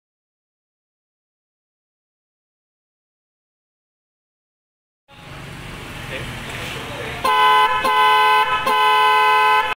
KLAKSON DCS BLACK TWIN TONE
Klakson DCS Black Twin Tone Universal
Spesifikasi:
- 400 / 500 Hz
- 110 dB(A)
- 12 V
- 82 mm
- ECE R28